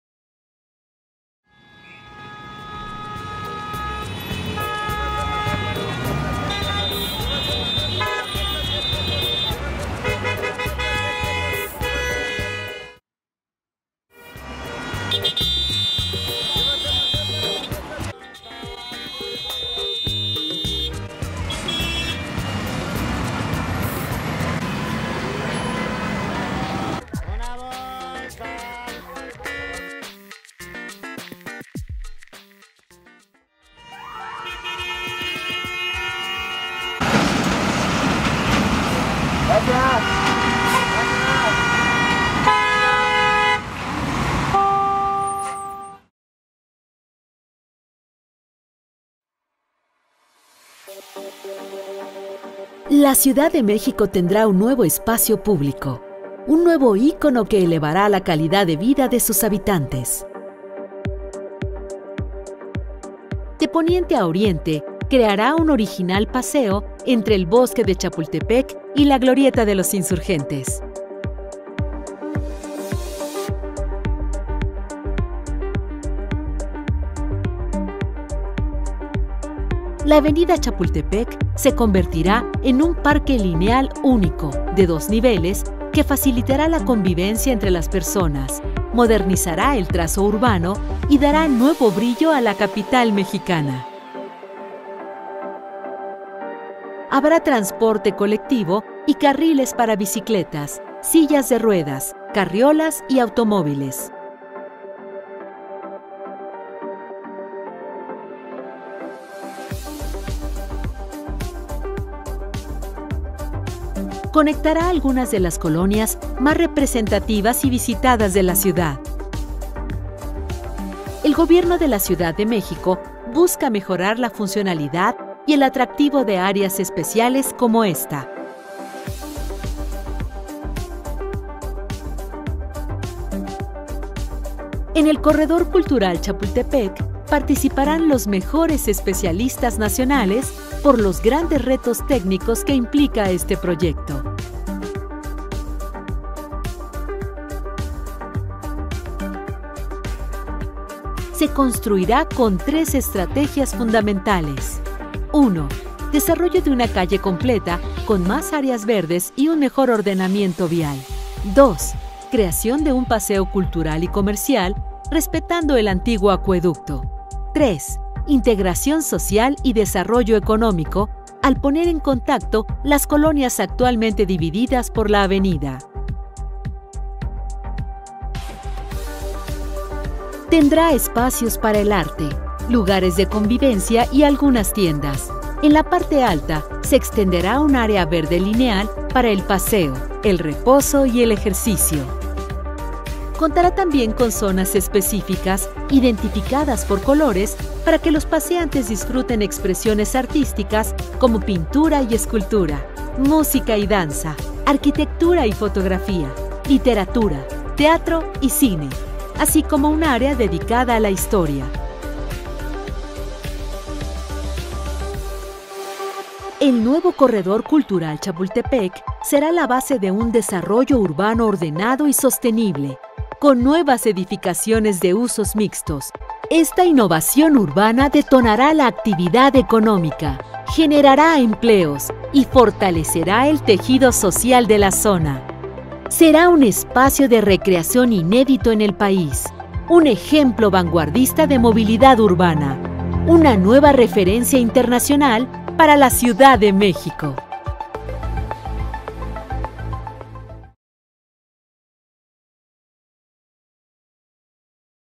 Mexican architects to create public park in the centre of a busy Mexico City road
Fernando Romero and two other Mexican architects will transform Mexico City's Avenida Chapultepec into a public park planted with trees and featuring cafe plazas and a raised promenade.